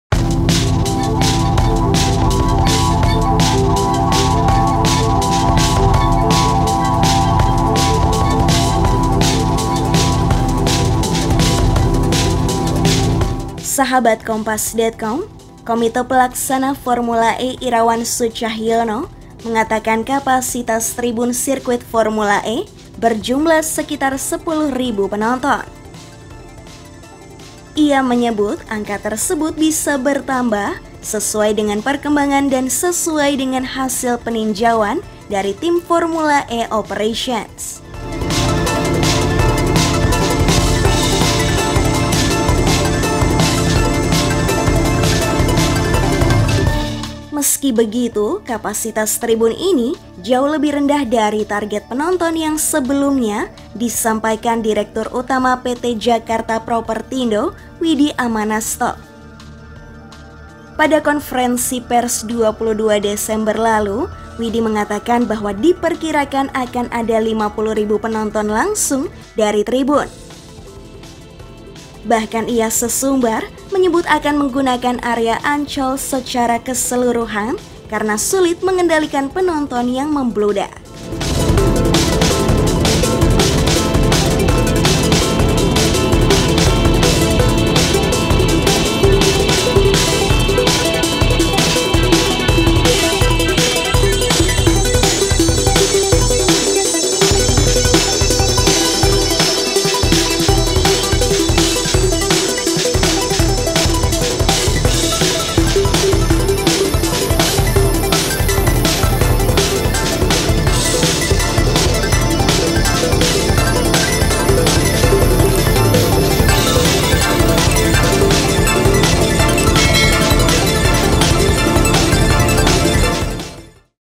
Target Penonton Langsung Formula E Jakarta Menyusut dari 50.000 Menjadi 10.000
Komite Pelaksana Formula E Irawan Sucahyono mengatakan kapasitas tribun sirkuit Formula E yang disediakan untuk penonton langsung berjumlah sekitar 10.000. Angka tersebut bisa bertambah sesuai dengan perkembangan dan sesuai dengan hasil peninjauan dari tim Formula E Operations.

Meski begitu, kapasitas tribun yang disediakan ini jauh lebih rendah dari target penonton yang sebelumnya disampaikan Direktur Utama PT Jakarta Propertindo Widi Amanasto. Pada konferensi pers 22 Desember lalu, Widi mengatakan bahwa diperkirakan akan ada 50.000 penonton yang langsung menyaksikan gelaran Formula E dari tribun.

Penulis: Singgih Wiryono
Penulis Naskah: Michaela Winda Saputra
Narator: Michaela Winda Saputra
Video Editor: Michaela Winda Saputra
Produser: Elizabeth Prillia Yahya Carvallo